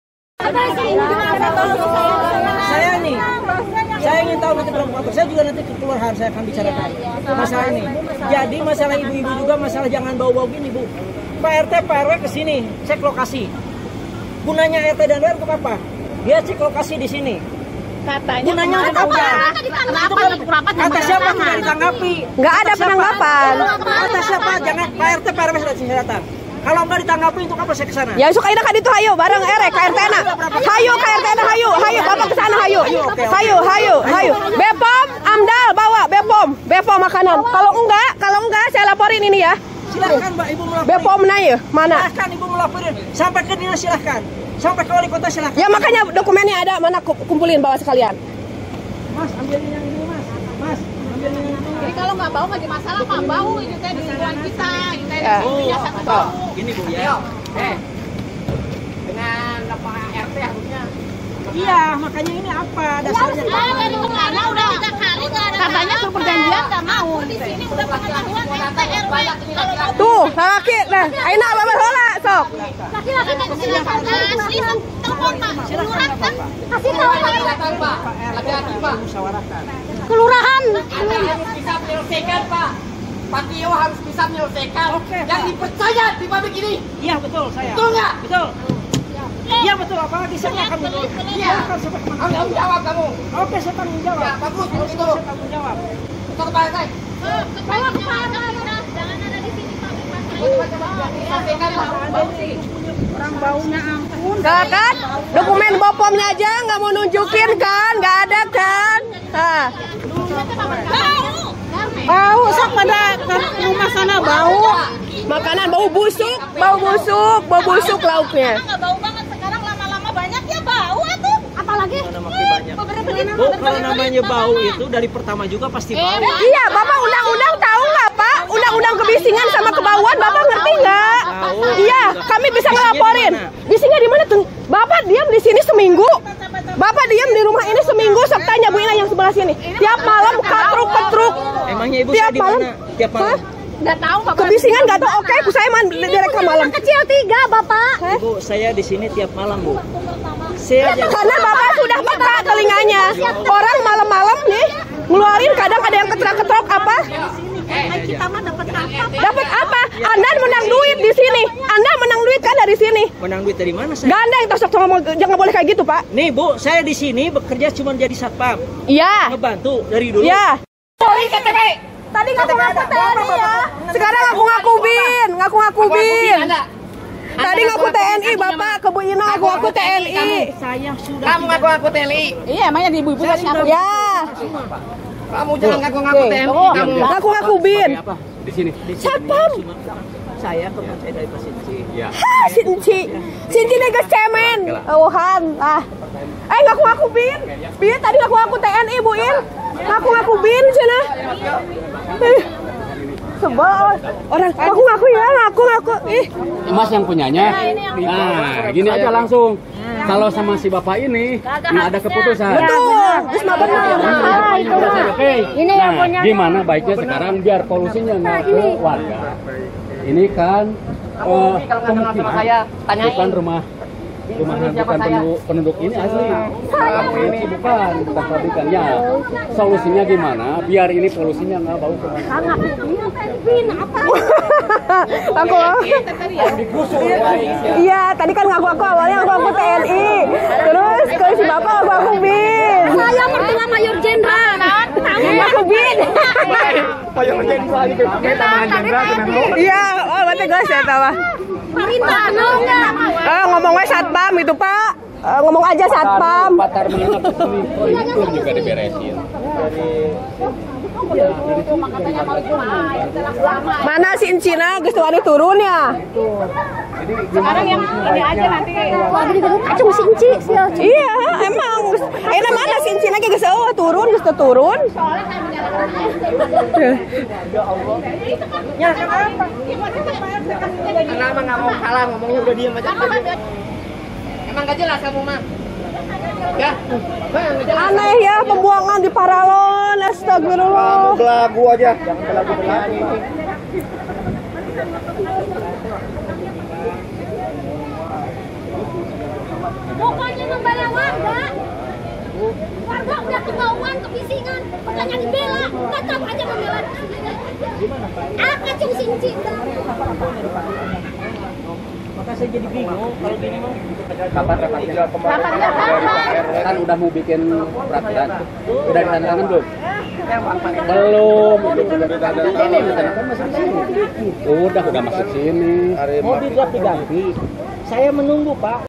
Mengaku BIN dan Anggota TNI Digerudug Ema-ema Karena Limbah
Seorang Satpam yang bekerja disebuah perusahaan? adu mulut dengan ema-ema gegara limbah dari adonan Basreng bau menyengat ke kawasan pemukiman.